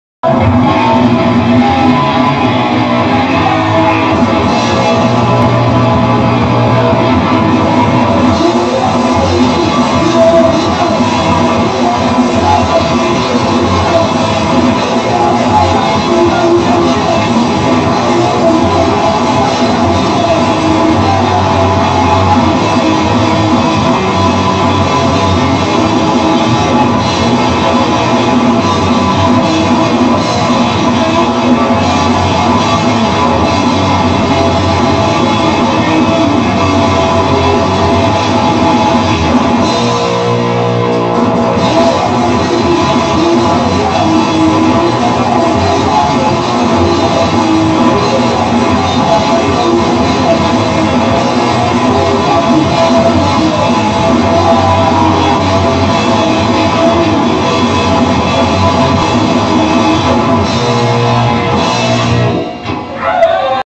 KILLSWITCH live 2001 pt. 6
KILLSWITCH live @ Changes on the strip in Lafayette, La. in 2001

JOSH " wolf" - vox

NATHAN - guitar

GREG - guitar

HOWIE - bass

DANIEL - drums